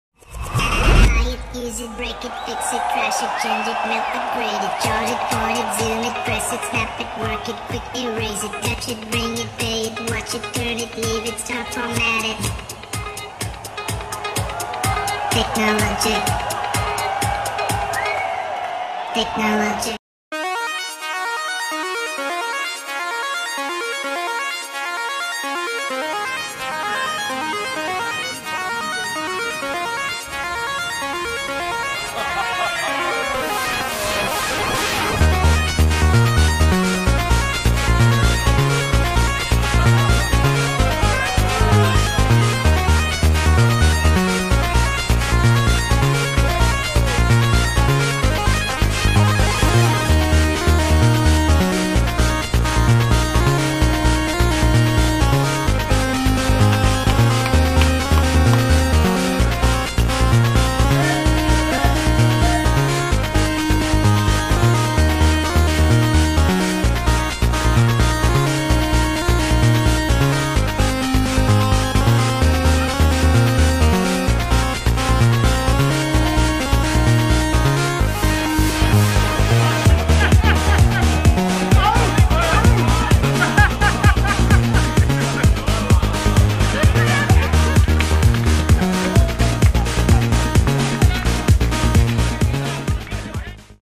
Lan-Nation v9 Funcontest Flieger
Sei es aus langeweile, Spaß an der Freude oder einfach nur so: Ein paar Jungs und Mädls haben auf der Lan-Nation v9 einen kleinen Flieger-Contest gestartet.